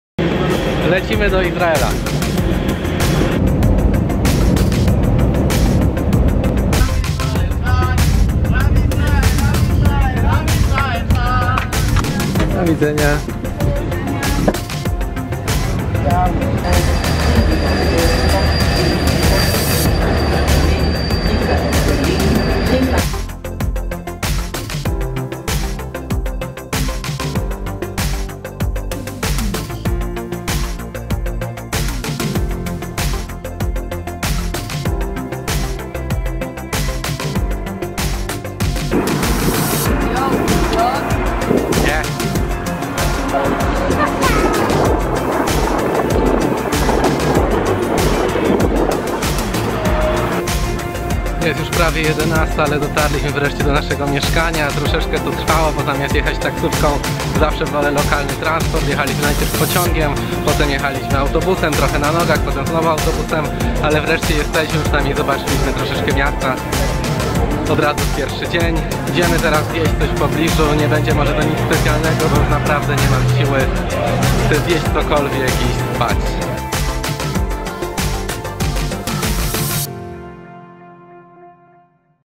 Lecimy do Tel Avivu - Izrael - #1 Daily Vlog
Wycieczka do Izraela w poszukiwaniu prawdziwego smaku hummusu, falafali, szakszuki oraz innych Izraelskich przysmaków.

Dzięki, Michał!